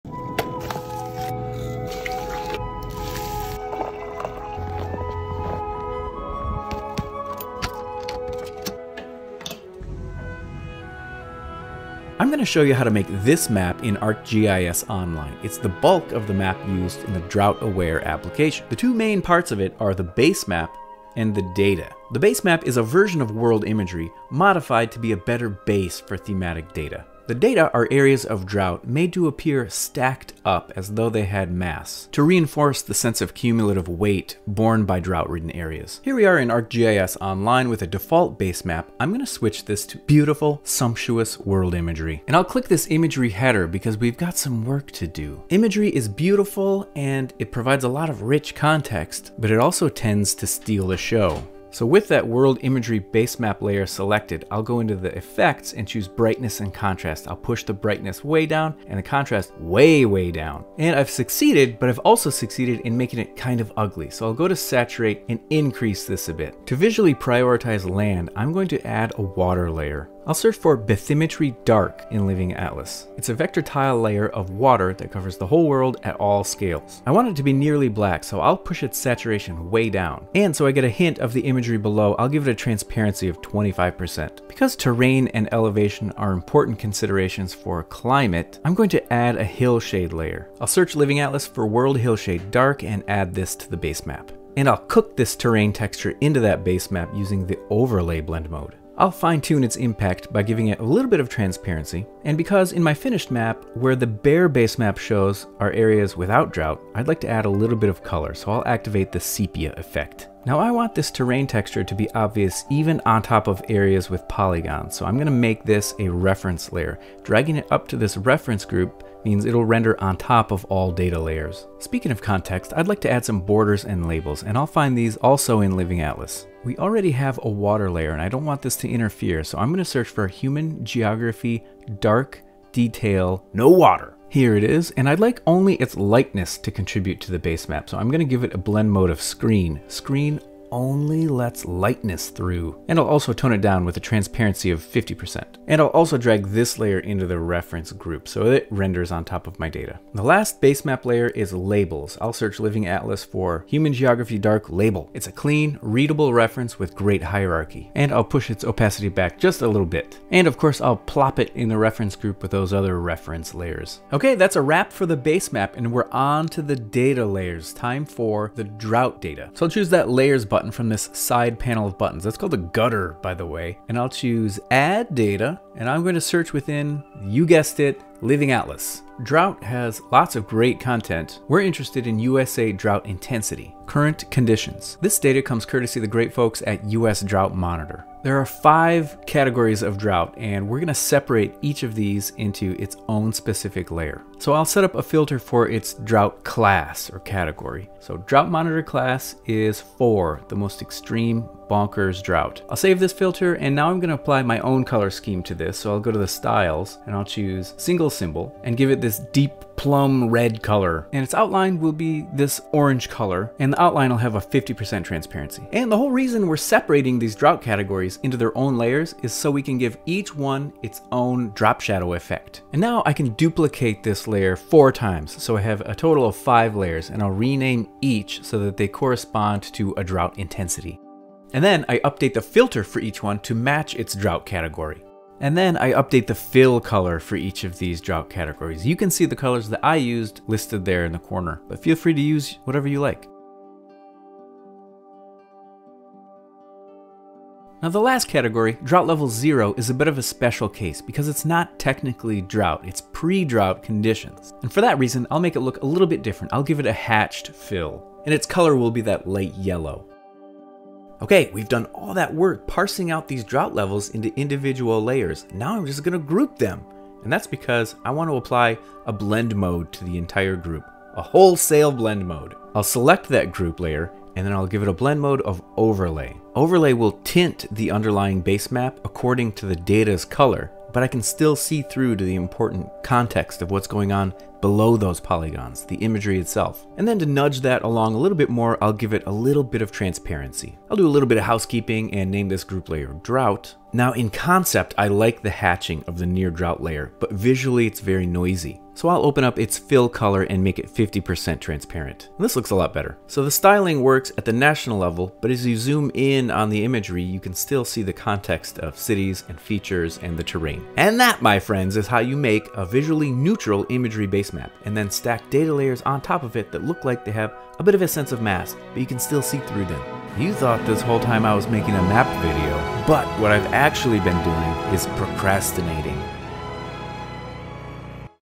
How to Make this Drought Map in ArcGIS Online
Here's how to make the drought map used in the Drought Aware app. This video covers creating a custom imagery basemap, modified to best support thematic data, and a styling method for the overlain drought polygons so they tint the imagery a color based on severity and have a cumulative shadow appearance to give a sense of mass.

0:00 Cozy caffeinated introduction
0:36 World Imagery modified to be a better thematic base
2:01 Borders and labels
2:47 Parsing and styling drought data
4:20 Blending the drought into the map
5:20 Driveway maintenance